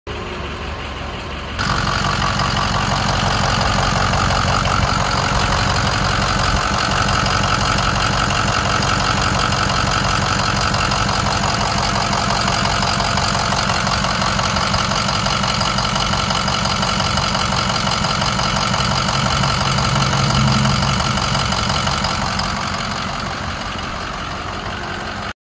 2007 Ford F550 Super Duty XL SuperCab flatbed truck at auction | bidding closes January 26, 2022
2007 Ford F550 Super Duty XL  SuperCab flatbed truck
 VIN: 1FDAX57P77EA61743
Engine
 Displacement: 6.0L V8 OHV 32V
 Cylinders: 8
 Fuel type: Diesel
Transmission
 Automatic
 Four wheel drive
Interior
 AC, Heat
 Cruise control
Operational
 Flatbed
 11' L x 94" W
 Removable sides: 24"
Tires
 Size: 225/70R19.5
Notes
 Cylinder heads studded
Missouri title